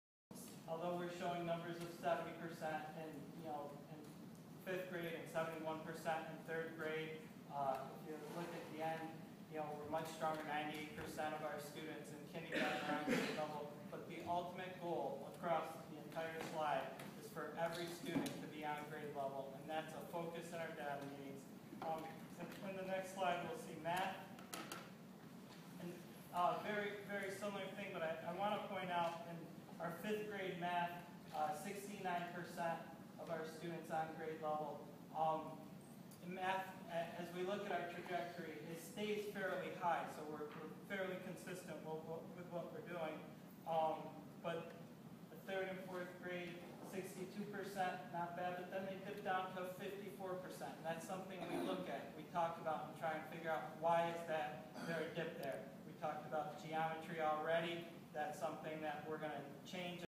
Principal and staff present on Ballston Spa's Wood Road Elementary School at the board of education
Principal and staff present on Ballston Spa's Wood Road Elementary School at the board of education meeting.

By: Jennie Grey
Published on: June 19, 2014